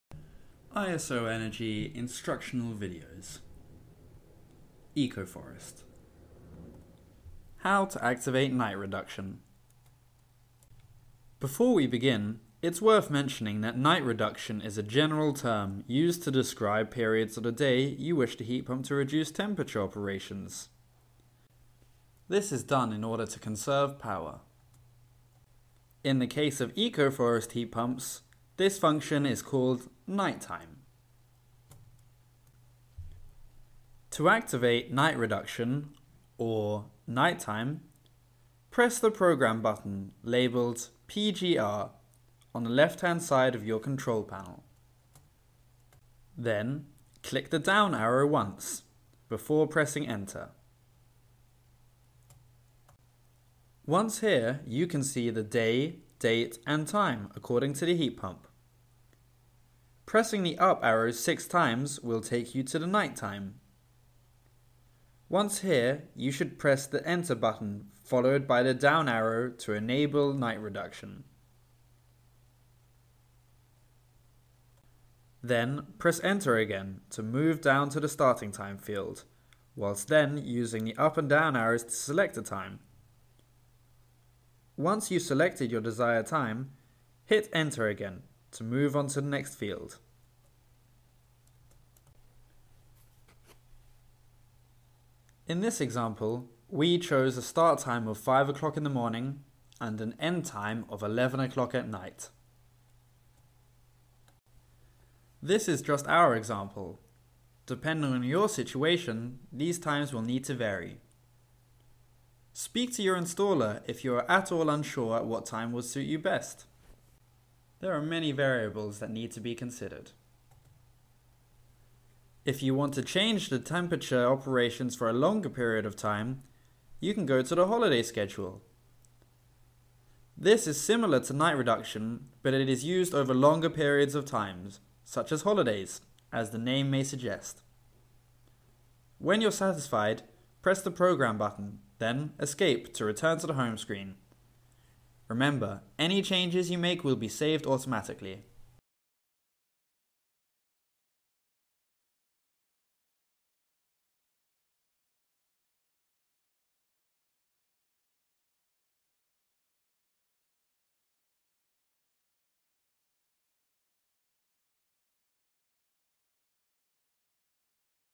isoenergy instructional Ecoforest video - Heat pump night reduction
How to use night reduction on an Ecoforest heat pump.
Video and voiceover by Joakim Broadbent